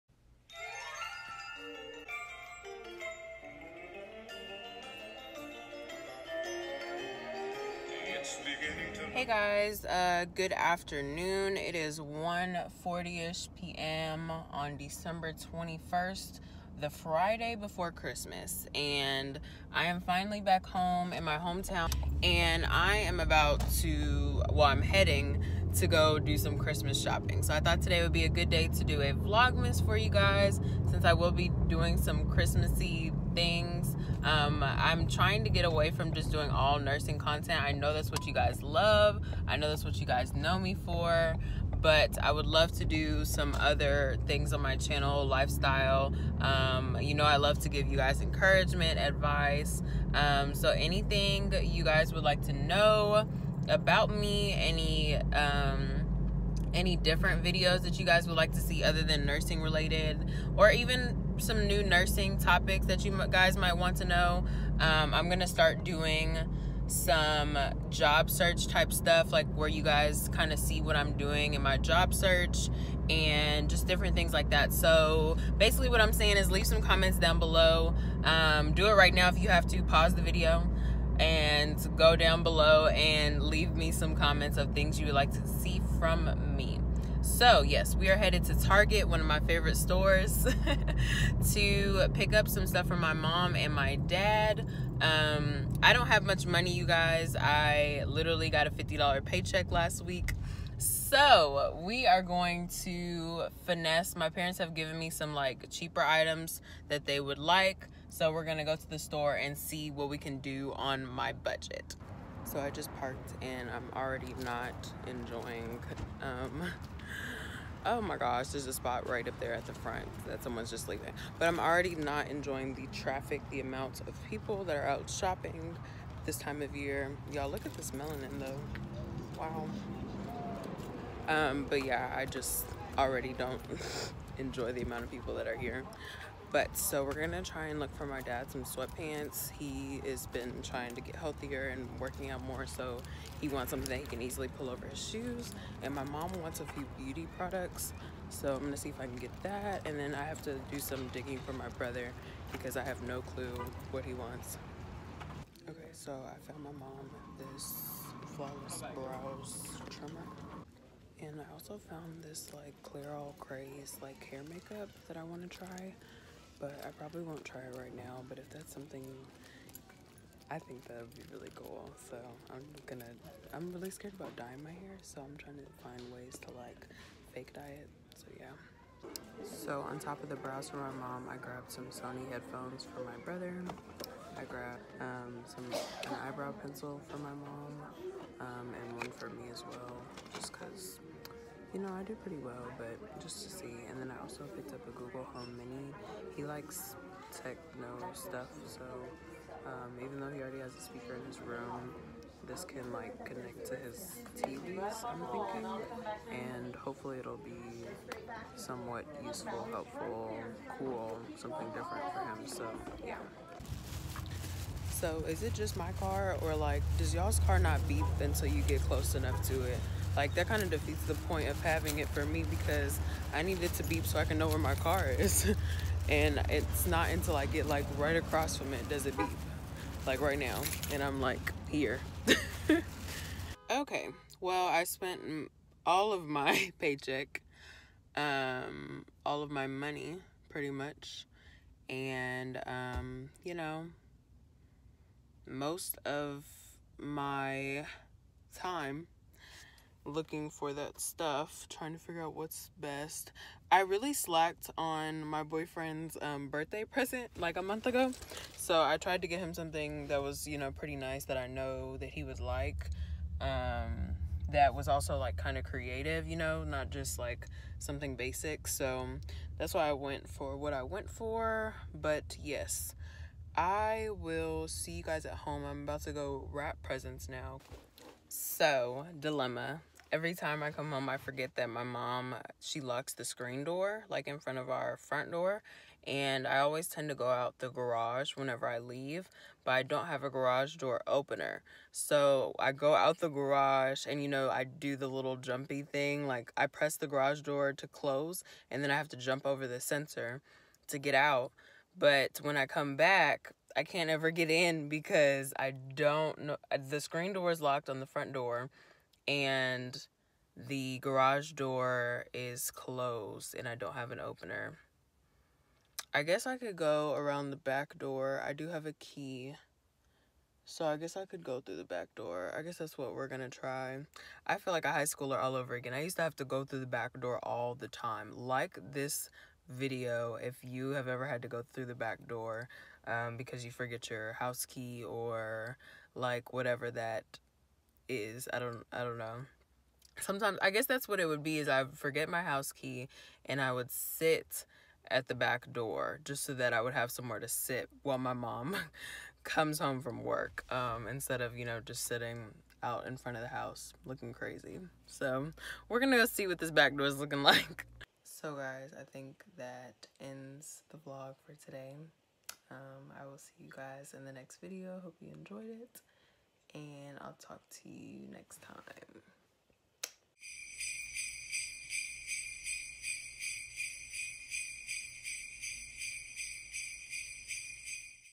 VLOGMAS DAY 22 | LAST MINUTE CHRISTMAS SHOPPING AT TARGET; WITH A BUDGET!
This video is where I went to Target and did my last-minute christmas shopping and tried to find budget friendly items. Im sorry I havent been consistent with vlogmas, and hope you guys will forgive me, thank you